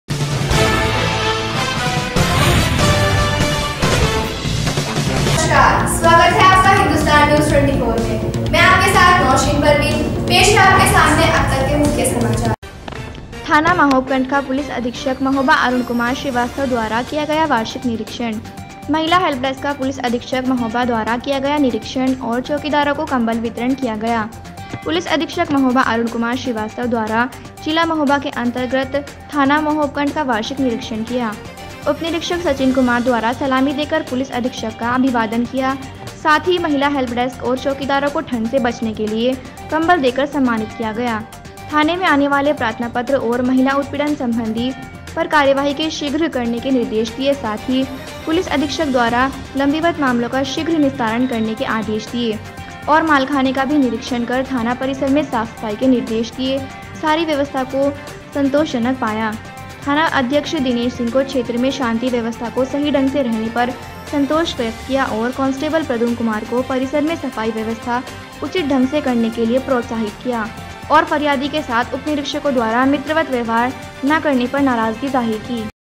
महिला हेल्प डेस्क का पुलिस अधीक्षक महोबा ने किया निरीक्षण और चौकीदारों को कम्बल वितरण किया गया
महोबा ब्यूरो चीफ संतोष कौशिक कि रिपोर्ट